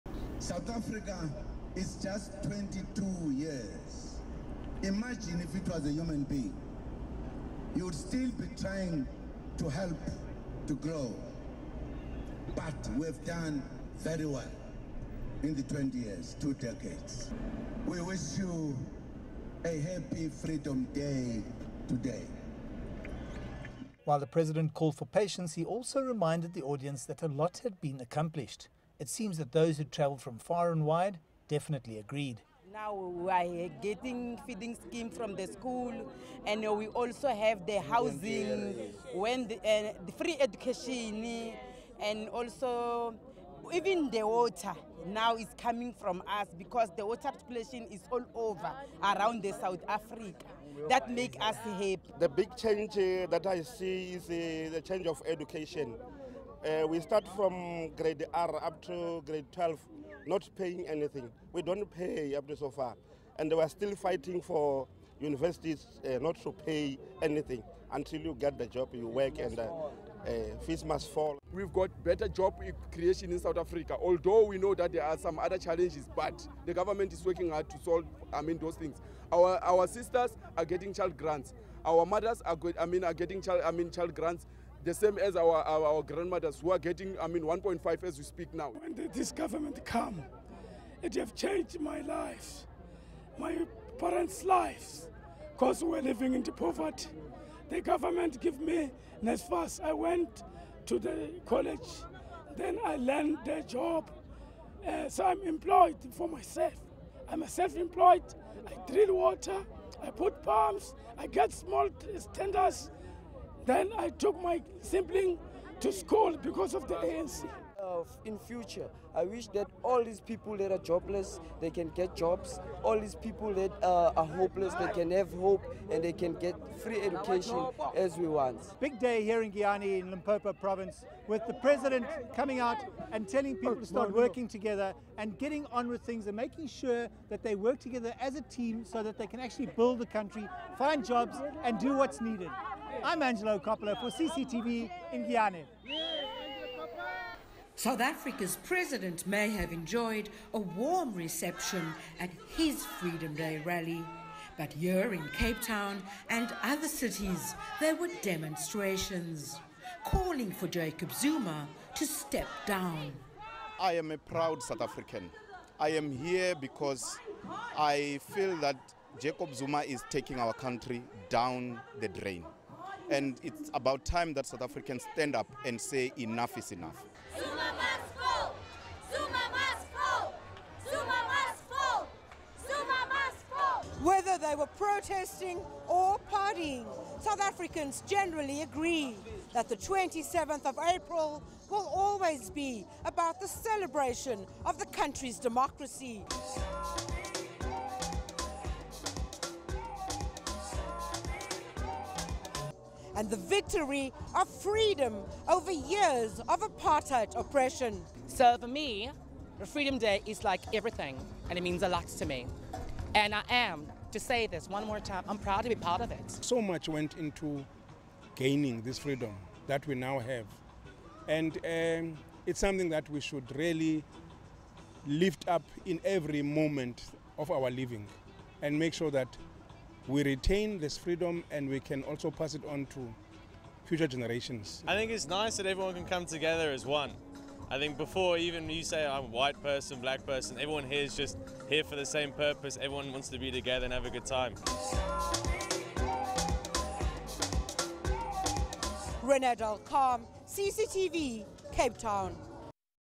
South Africa Freedom day official celebrations in Limpopo
South Africa celebrated Freedom Day, with the official celebrations taking place in Giyani, in the Limpopo province. The area was declared a disaster zone several years ago and the government intervened and have declared it a flagship region. President Zuma, and his deputy Cyril Ramaphosa and several other cabinet ministers joined him. Angelo Coppola reports